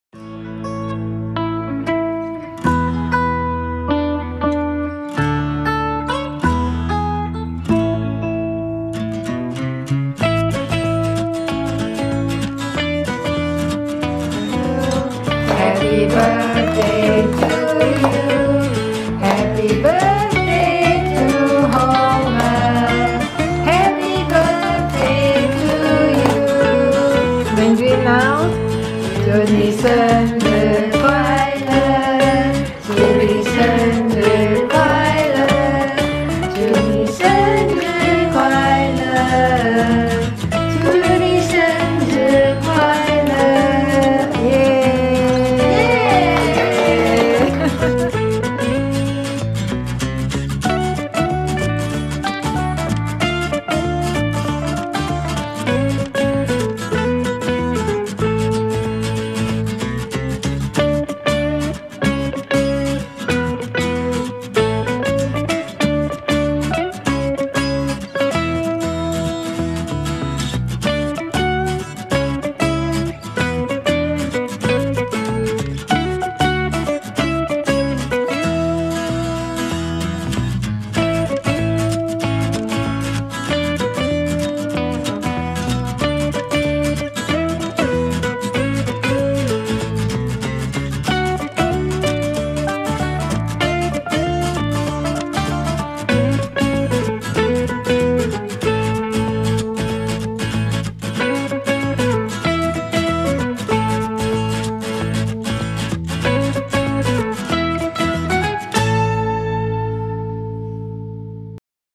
Mini Schnauzer's 14th birthday 2nd April 2021
Homer the mini schnauzer ( the nearly April fool boy!) celebrated his 14th birthday with a homemade cake that made him drool. But before he could taste it, he had to endure the mandatory birthday song..
We are grateful that Homer has remained relatively healthy, as well as his age allows him to be. We wish him many, many more happy returns.